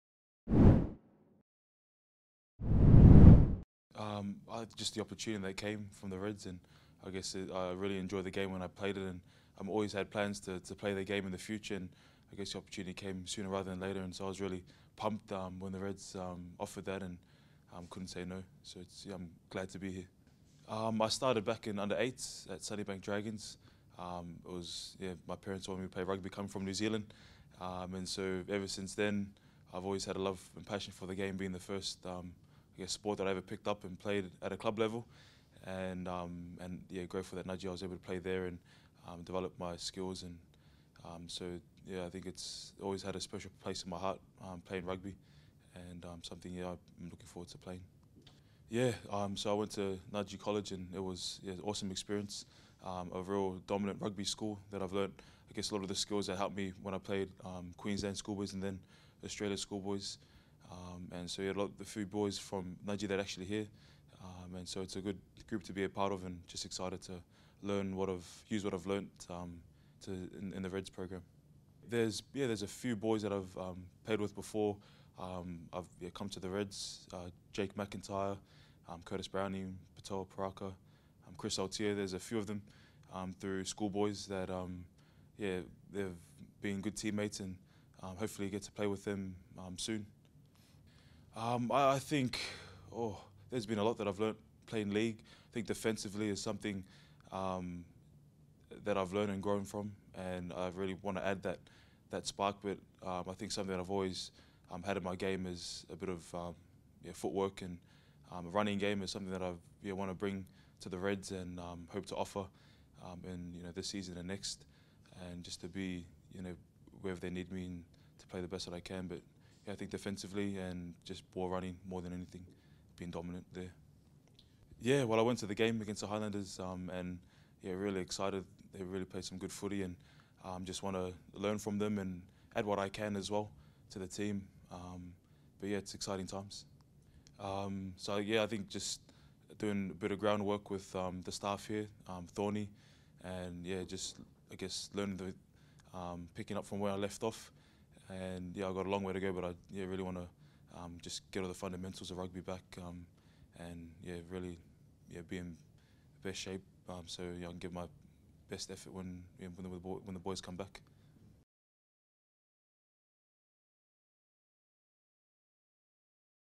Caleb Timu joins the Reds
The St.George Queensland Reds have signed Broncos forward Caleb Timu through to the end of 2017.

The 22-year-old will join the Reds for training at Ballymore immediately, after seeking an early release from his contract with the NRL club.